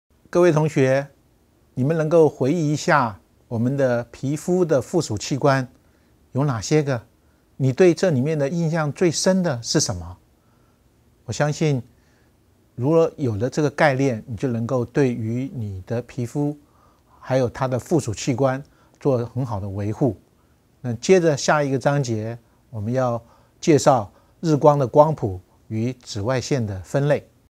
紫外線與防曬-03總結
課程簡介：介紹完皮膚的附屬器官後，你能記得那些呢並且能應用在生活日常中。
適合對象：對化妝品、藥妝品有興趣或是有志了解化妝品、藥妝品的同學均可適用，對曾經使用過化妝品者或曾在大專校修習相關課程者在學習上會更加容易。
授課教師：徐令儀
製作單位：中華科技大學 遠距教學組
製作人員：林文博 郭恬君